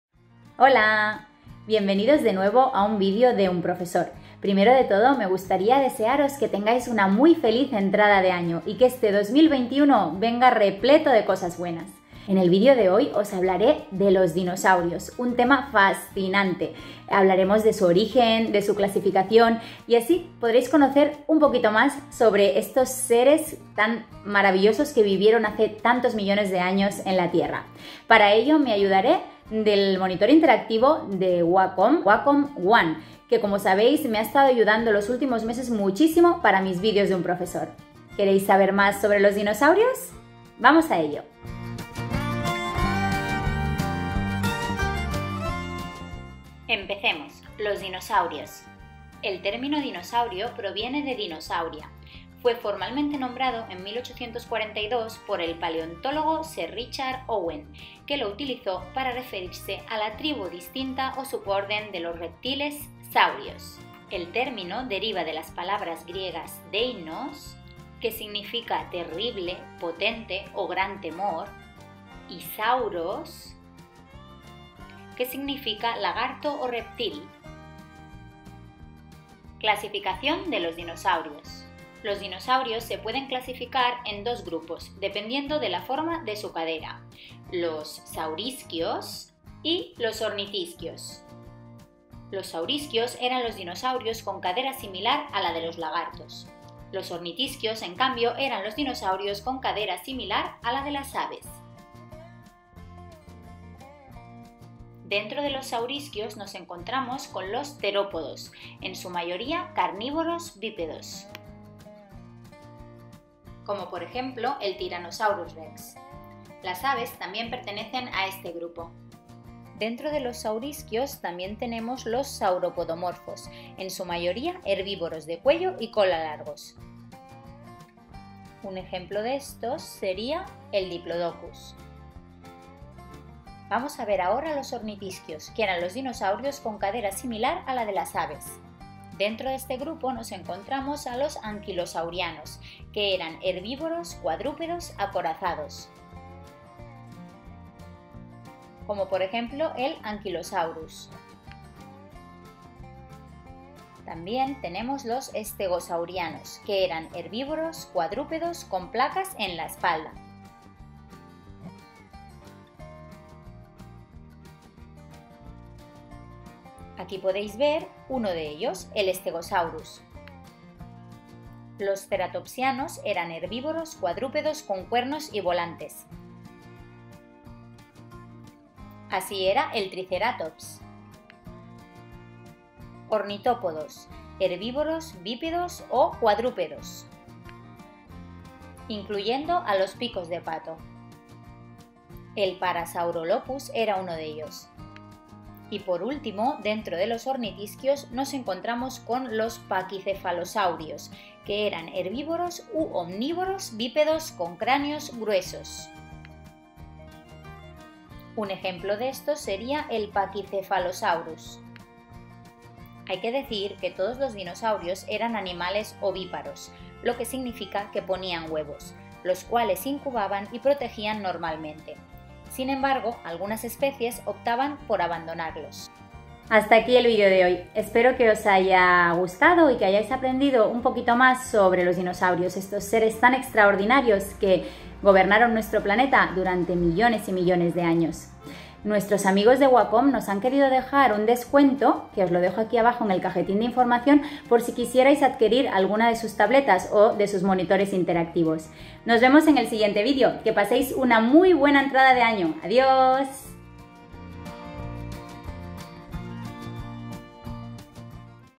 Los DINOSAURIOS: Origen y clasificación 🦖 (RESUMEN)
Aprende en este vídeo QUÉ TIPO DE DINOSAURIOS EXISTIERON y cómo los clasificamos. Hablaremos de todos ellos con interesantes datos de los dinosaurios PARA NIÑOS pero también para todo el que quiera volver a REPASAR y saber cual es hasta la fecha la CLASIFICACIÓN de dinosaurios para niños en ESPAÑOL.